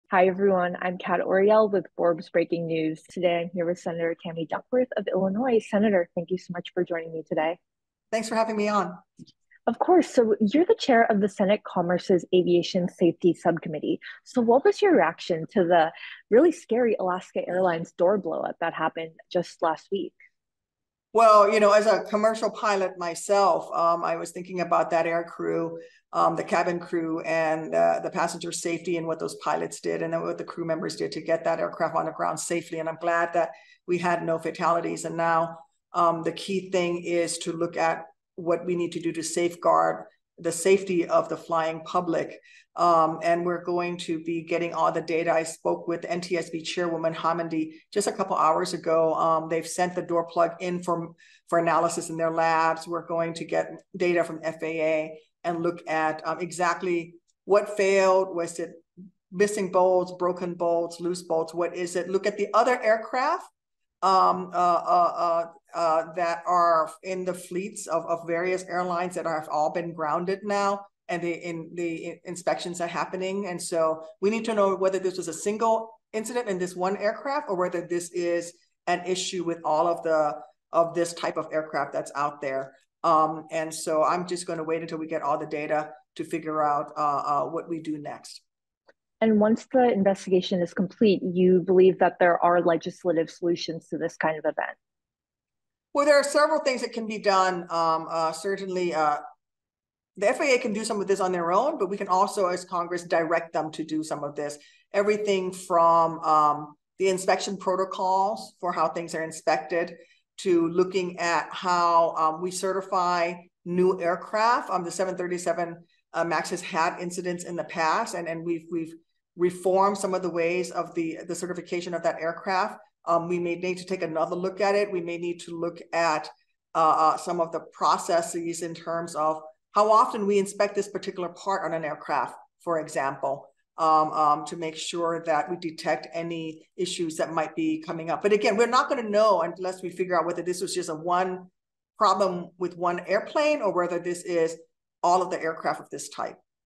Tammy Duckworth Calls For Legislative Solutions After Door Plug Falls Off Boeing Plane
On "Forbes Newsroom," Sen. Tammy Duckworth (D-IL), who is a civilian pilot herself, reacted to the Alaska Airlines Flight 1282 door blowout, what legislative steps should be taken, and what the next steps are.